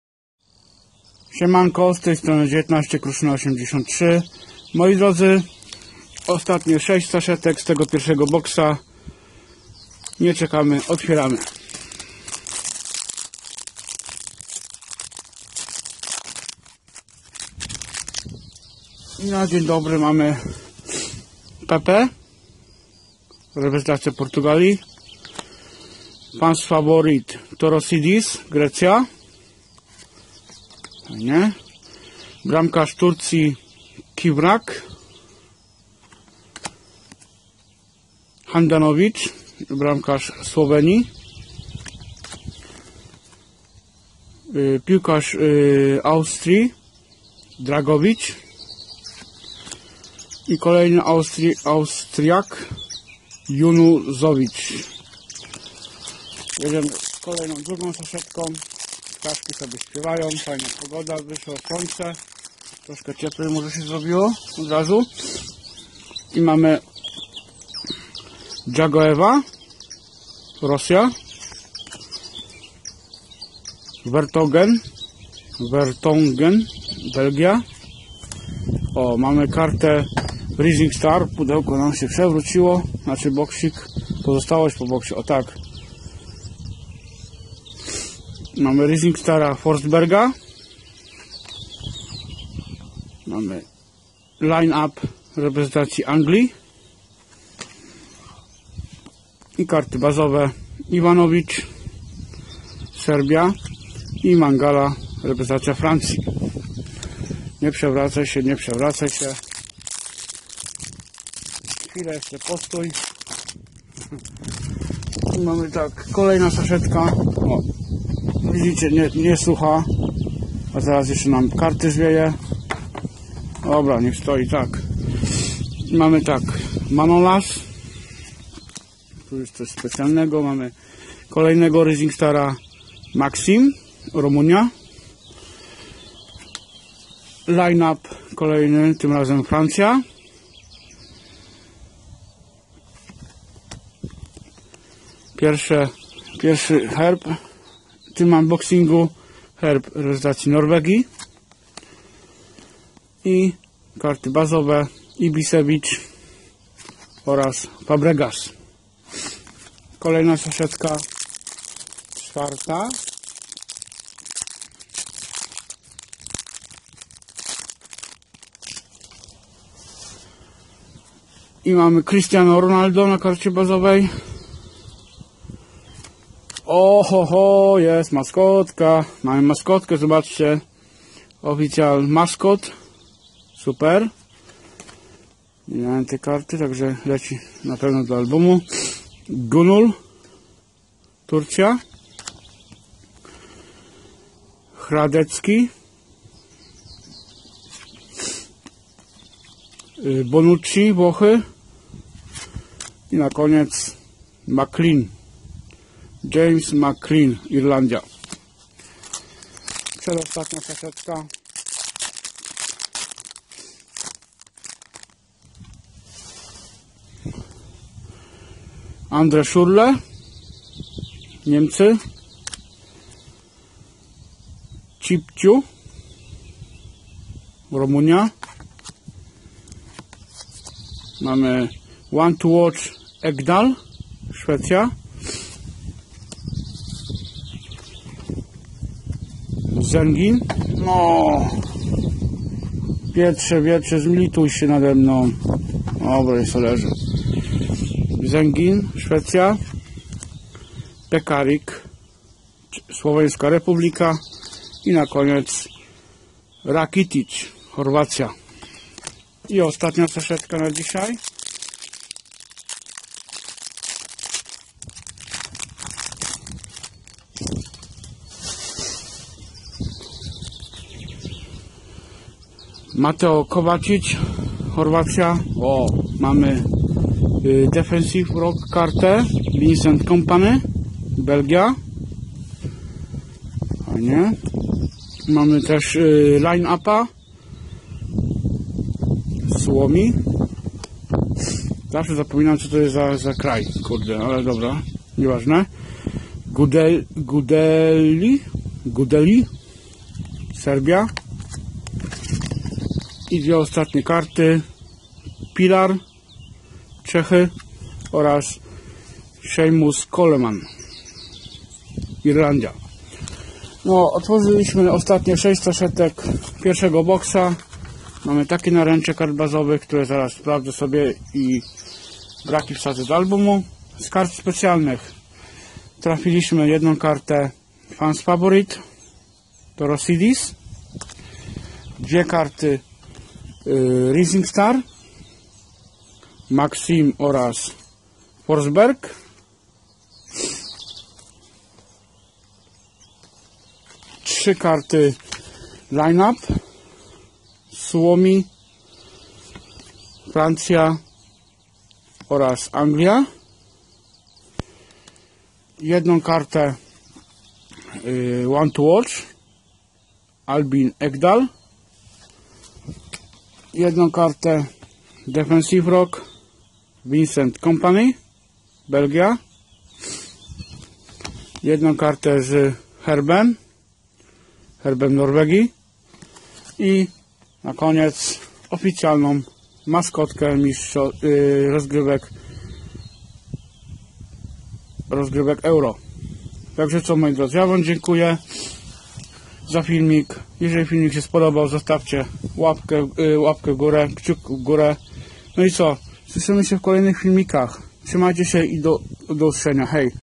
BOX #1 - PANINI XL ROAD TO UEFA EURO 2016 - UNBOXING BOX CZEŚĆ 4
Ostatnia czesc unboxingu boksa #1 PANINI XL ROAD TO UEFA EURO 2016